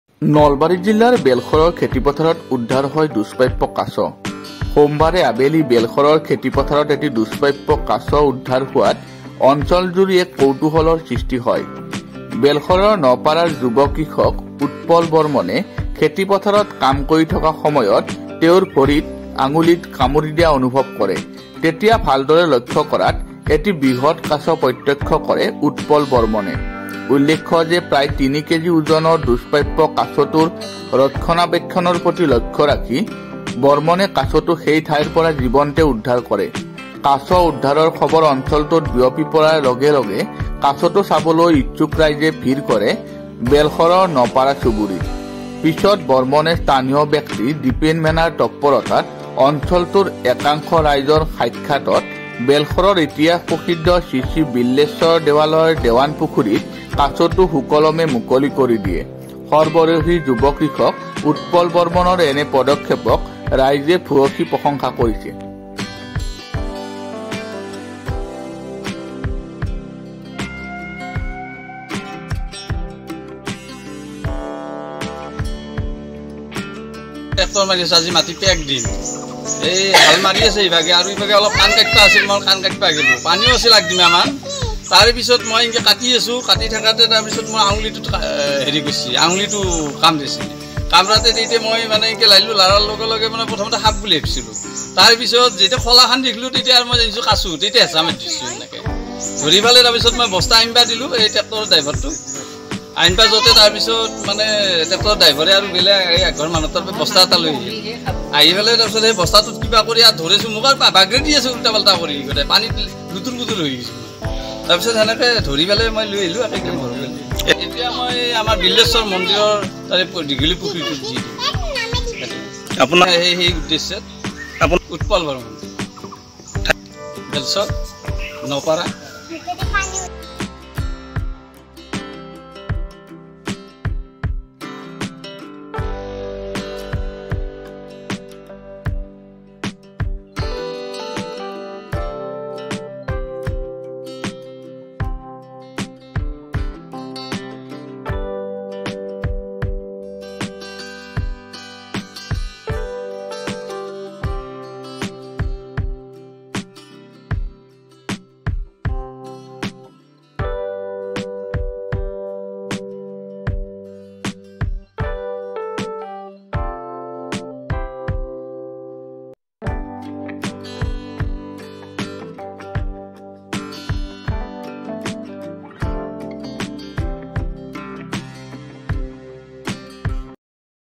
Turtle Found in Nalbari | পথাৰত উদ্ধাৰ হোৱা দুষ্প্ৰাপ্য কাছ বেলশৰৰ দেৱান পুখুৰীত মুকলি
Turtle Found in Nalbari | বেলশৰৰ খেতিপথাৰত উদ্ধাৰ হোৱা দুষ্প্ৰাপ্য কাছ বেলশৰৰ দেৱান পুখুৰীত মুকলি |